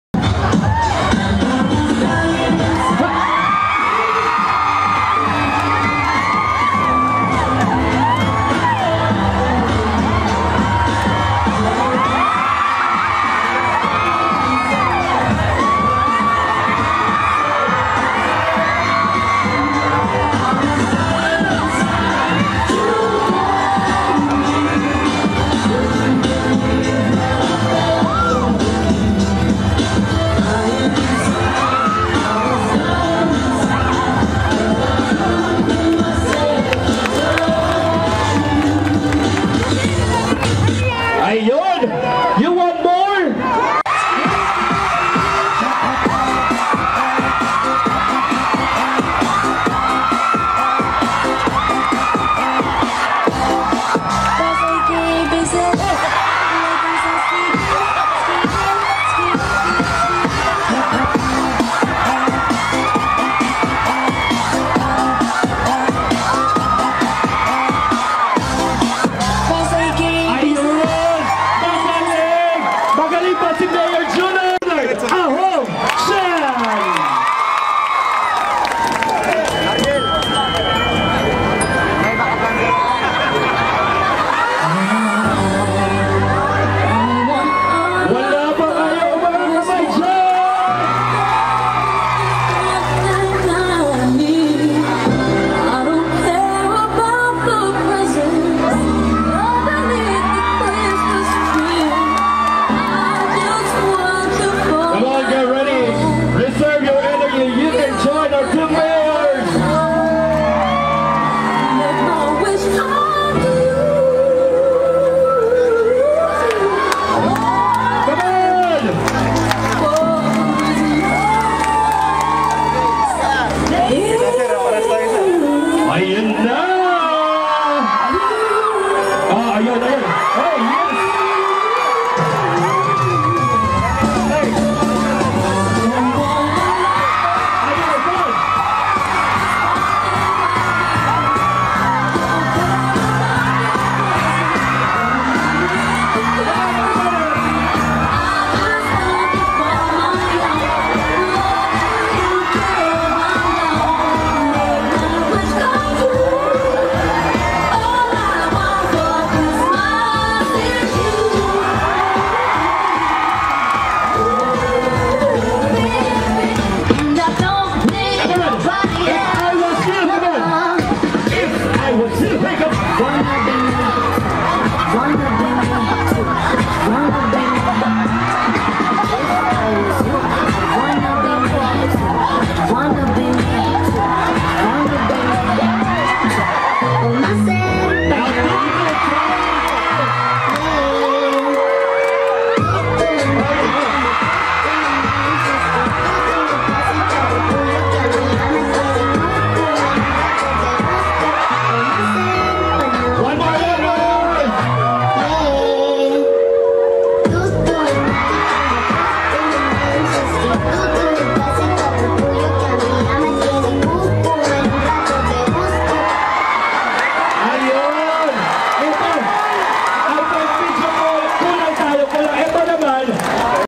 ISKO MORENO & MAYOR AHONG CHAN ( Dance Challenge )
Manila City Mayor and presidential aspirant Isko Moreno and Lapu-Lapu City Mayor Junard "Ahong" Chan accepted the challenge to have a Dance showcase on Monday morning, in front of the Lapu-lapu City hall, dated December 6, 2021.

2021 FSC Creatives and Productions
Cebu in Pictures

Render RedNose
Courtesy Junard Chan
2021 Cebu in Pictures